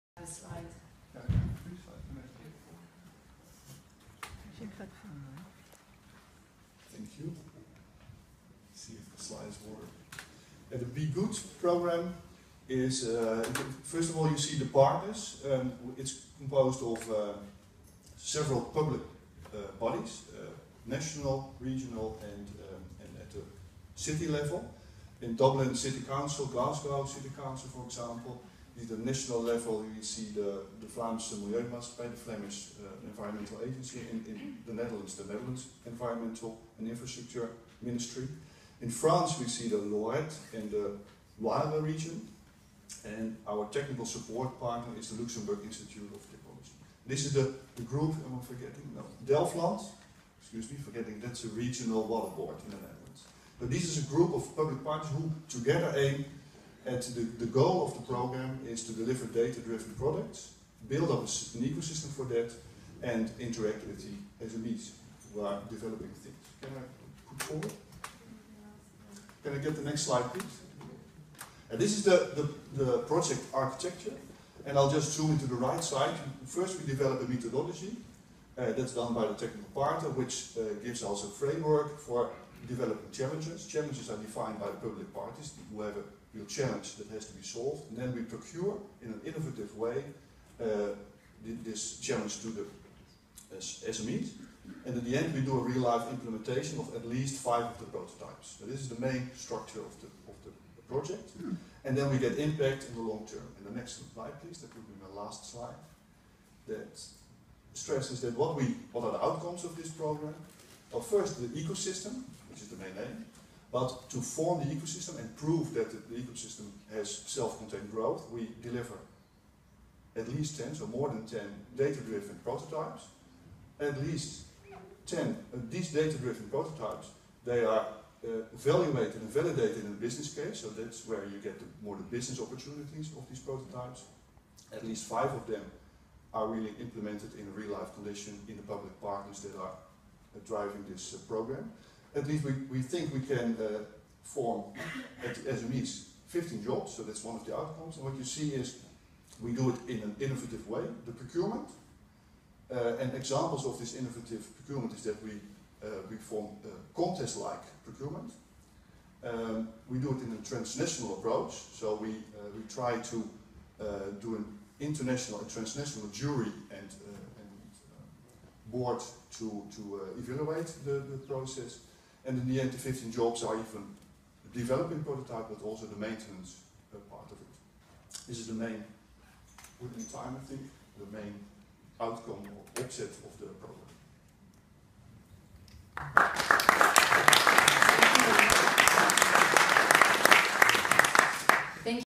The Be-Good Project
How would the partners of the BEGOOD project describe what they’re doing? From 2 June in Lille, France, a BEGOOD partner describes the partnership and expected outcomes of the project.
BE-GOOD is a pioneering project aiming to unlock, re-use and extract value from Public Sector Information (PSI) to develop data driven services in the area of infrastructure and environment.